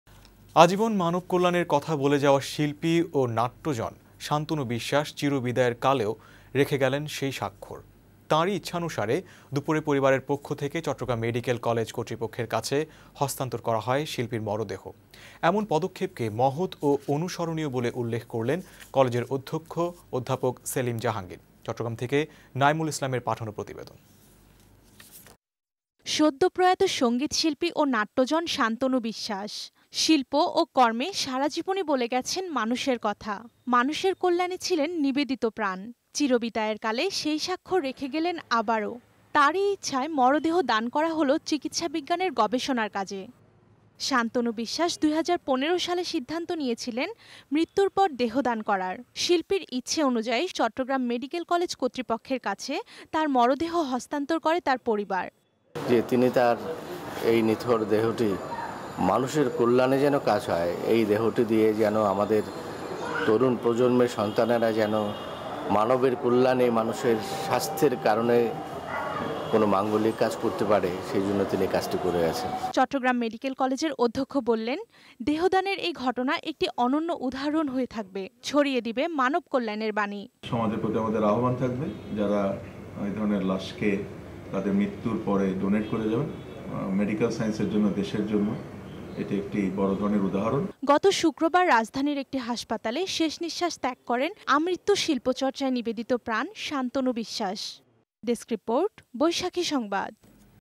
শিল্পী শান্তনু বিশ্বাসের মরদেহ চট্টগ্রাম মেডিকেল কলেজে দান | Shantanu Biswas | Latest News Of Bangla
শিল্পী শান্তনু বিশ্বাসের মরদেহ চট্টগ্রাম মেডিকেল কলেজে দান | Shantanu Biswas | Latest News Of Bangla | Tonima | 14Jul19